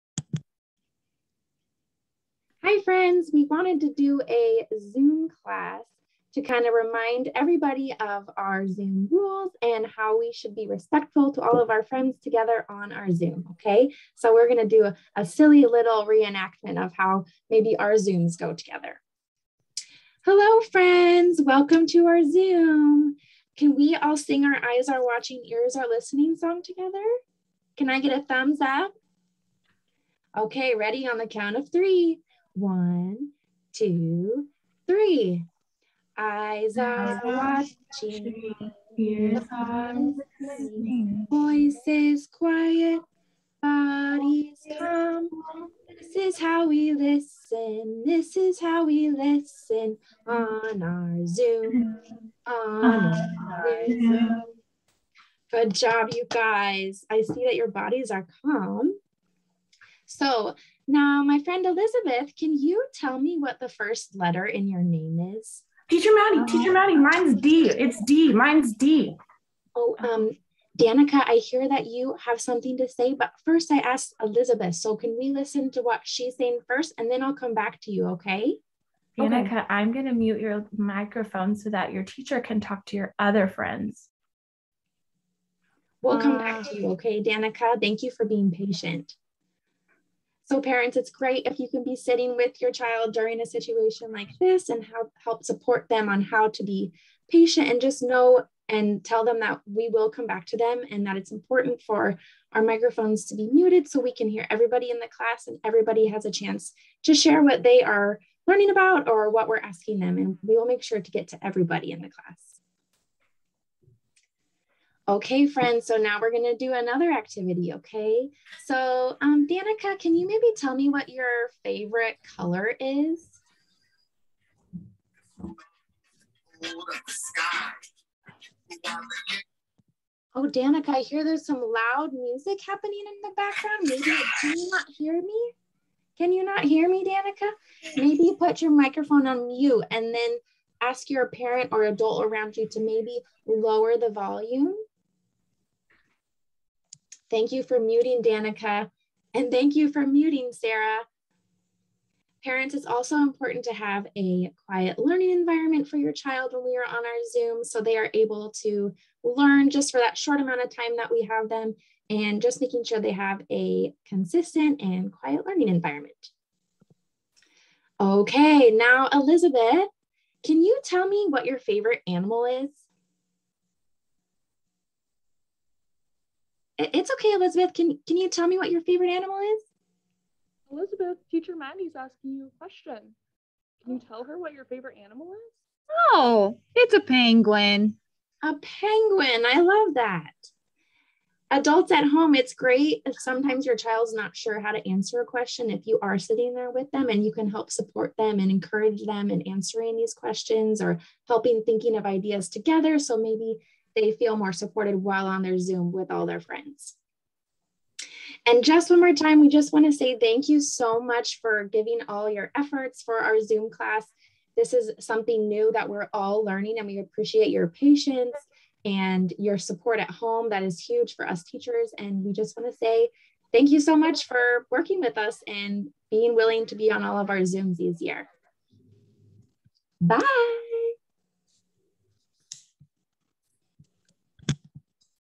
Zoom Class Reminders:)
Check out this video that your virtual teachers made together about how to be kind on our zoom!:)  We hope you like it!